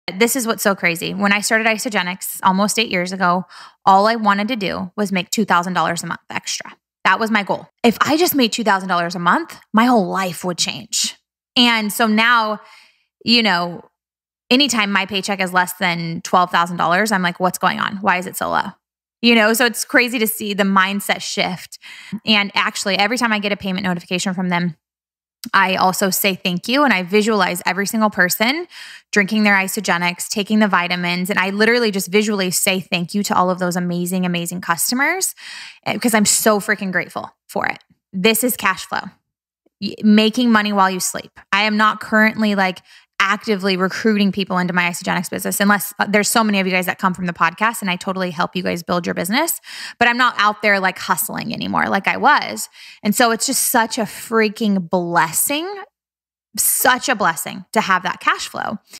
Cash Flow & Wealth - Ep.124
Today we’re talking about my two favorite words: cash flow!

You Will Hear About:
[1:45] Making money while you sleep
[3:30] What to do when you do have cash flow
[4:30] Acquiring assets, or something that passively earns you money or gains value
[7:10] The difference between financial freedom and cash flow
[9:30] How you can create more cash flow (or stop preventing it)
[19:00] Getting creative!

Resources:
Connect with Cayla: Facebook | Instagram

Want a chance to win a FREE pair of Lululemon pants? It’s easy!

Take a screenshot of your review

Facebook.com/CraftCayla

Instagram.com/Cayla.Craft

CaylaCraft.com